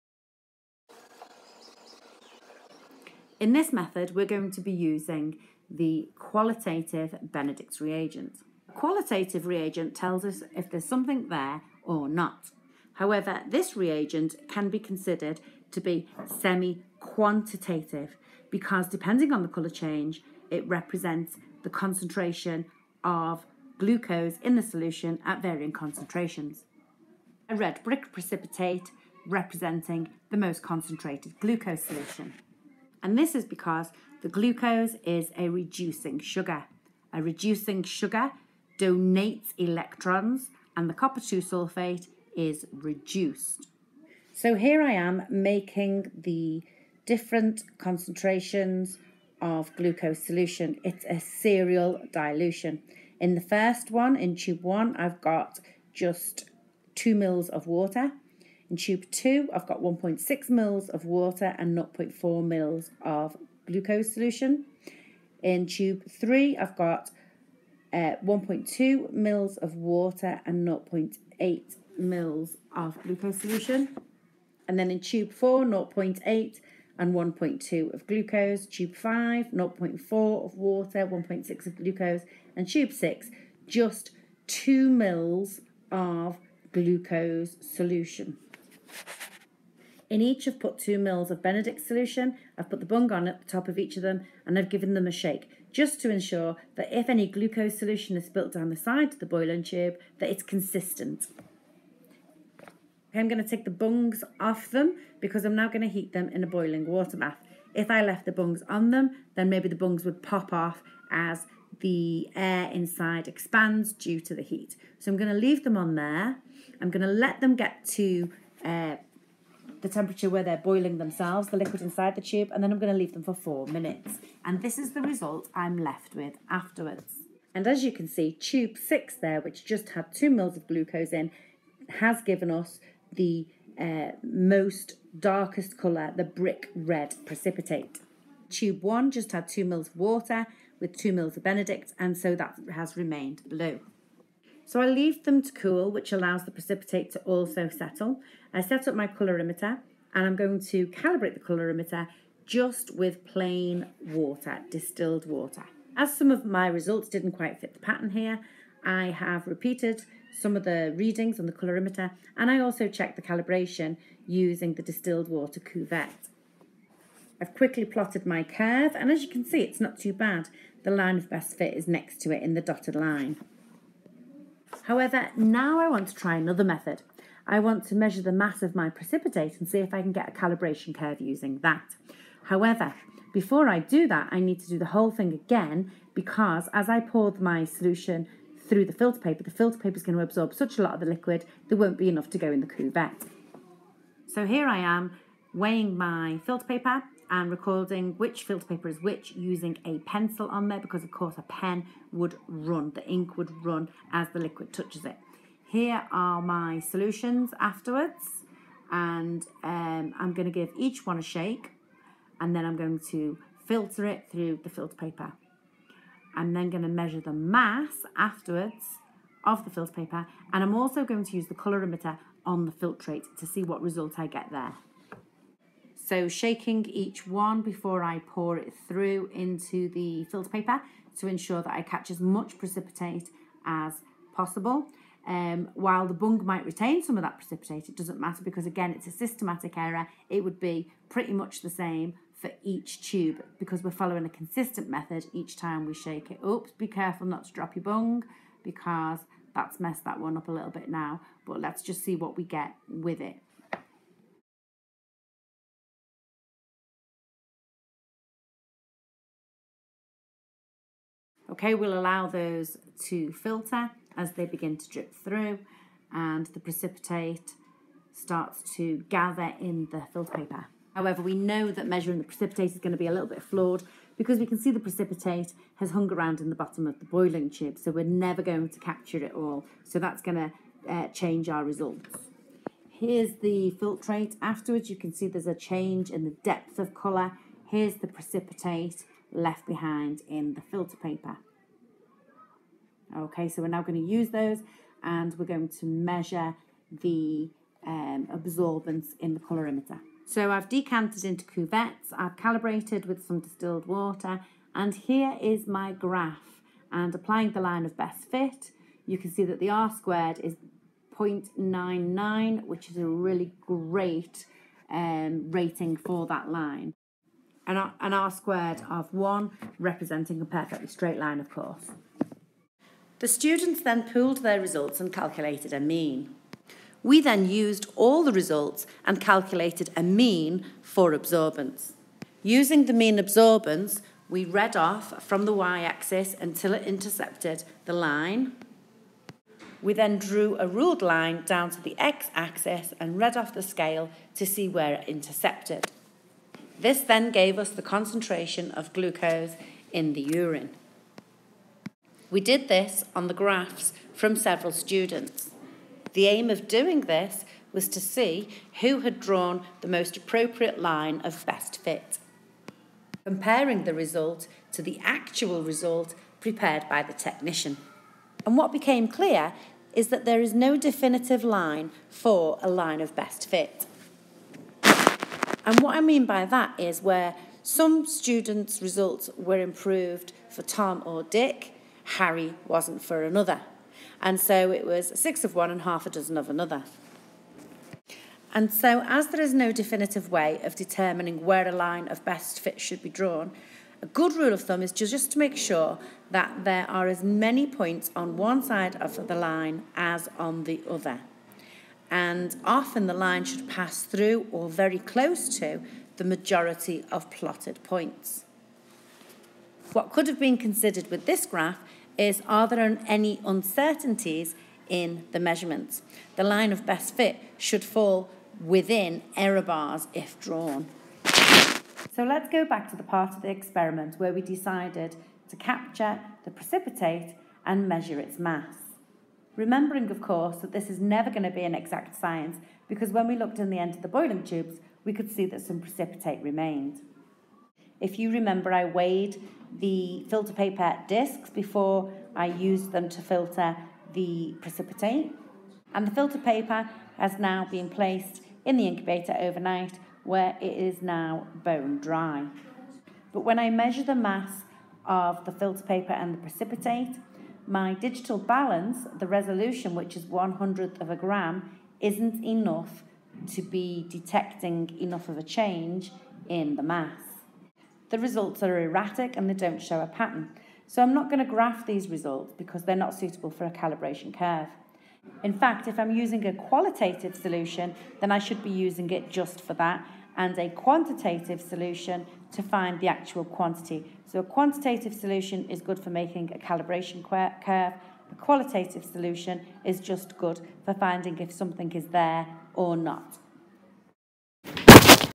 A level Required Practical 11 - a serial dilution, calibration curve
This practical explores and evaluates two methods of producing a glucose calibration curve using both the filtrate and precipitate produced when the solution was tested with qualitative Benedicts solution.  The calibration curve is plotted and the concentration of the unknown samples is read from the graph.  The line of best fit is evaluated as we use the student results to identify who drew the most appropriate line of best fit.  Weighing the precipitate to plot a calibration curve is dismissed